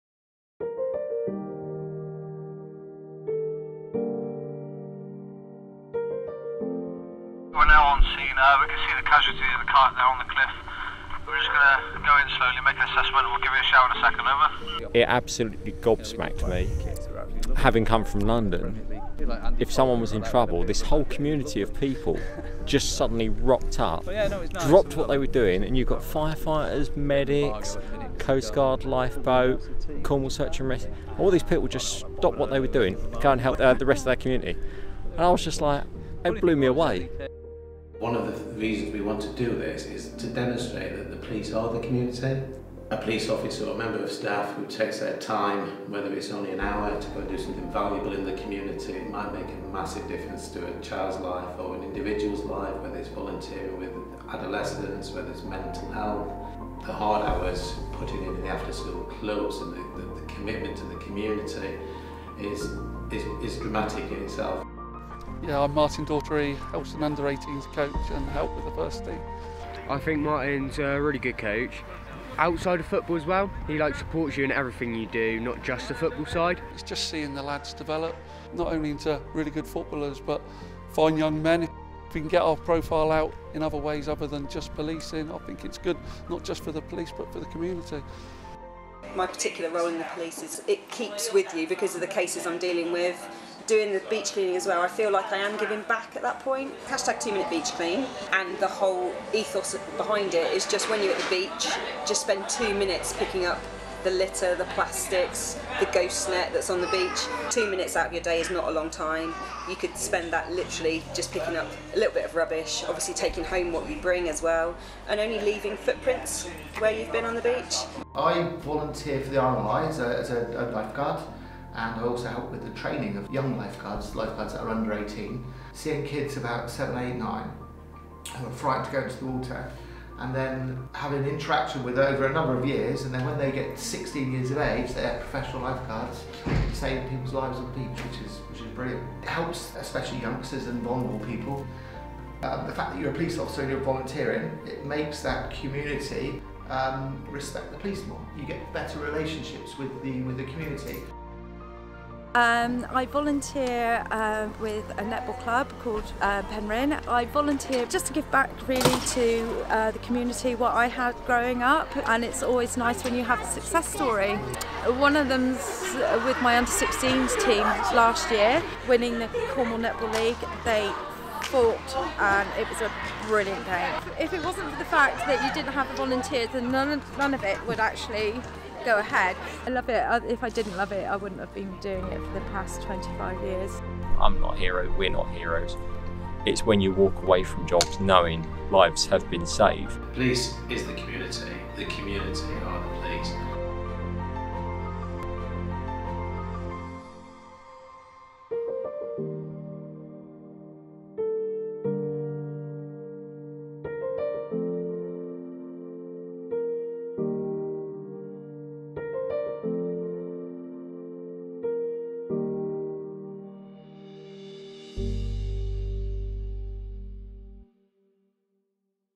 Policing is more than just a job
Devon & Cornwall Police celebrate the diversity and contribution police officers and staff make in their communities outside their formal roles.